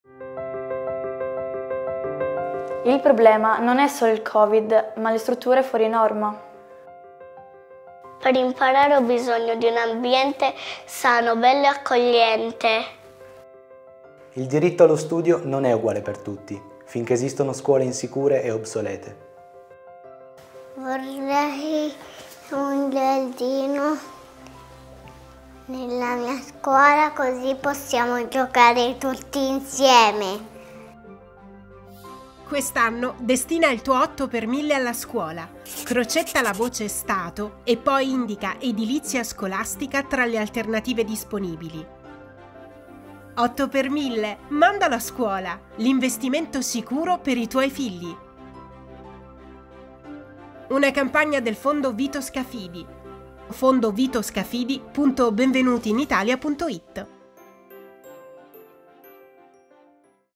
8*1000, mandalo a scuola (Spot 2)
Nella prossima dichiarazione dei redditi, crocetta la voce “Stato” e poi scegli “edilizia scolastica” tra le alternative disponibili.
Un gesto che garantisce un un investimento sicuro per i tuoi figli. Senza alcun costo aggiuntivo.
Una campagna del Fondo Vito Scafidi, maggiori informazioni su fondovitoscafidi.benvenutiinitalia.it